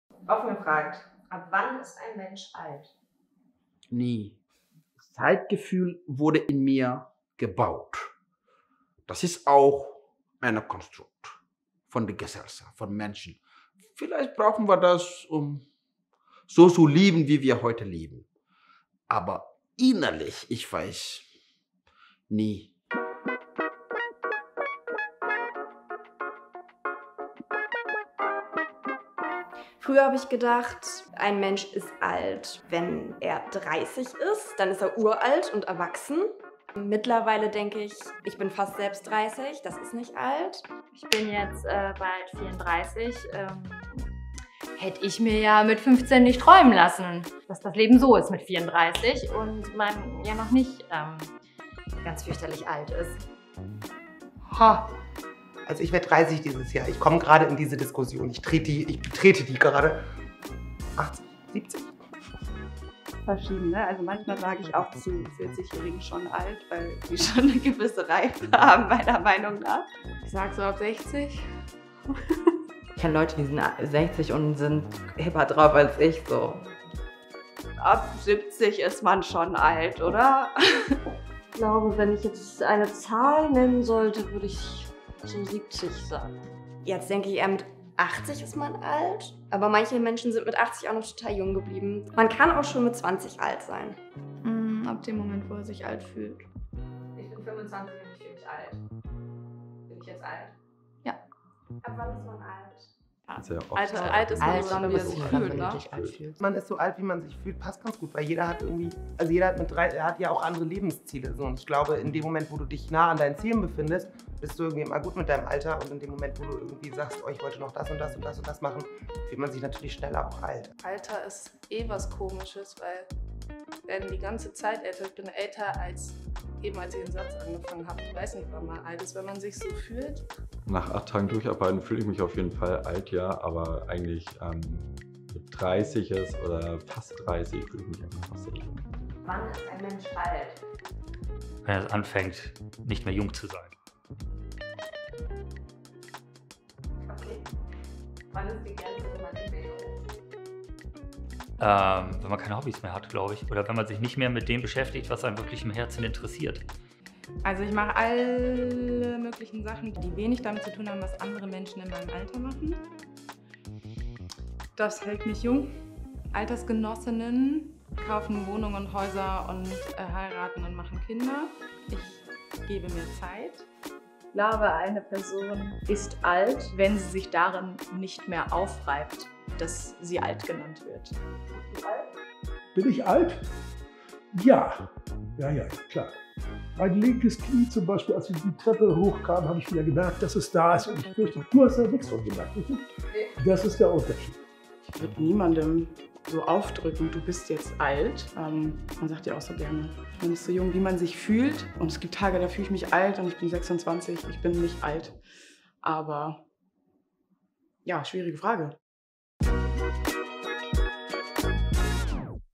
Offen gefragt Folge 1: Übers Altern
Ihr wisst es bereits: wir lieben es, Euch Fragen zu stellen. Wir glauben, dass Worte verbinden können und es noch nie wichtiger war, einander aufrichtig zuzuhören. Dieses Format liefert einige Antworten interessanter Persönlichkeiten auf spannende Fragen.

In Folge 1 haben wir haben offen gefragt: Ab wann ist ein Mensch alt?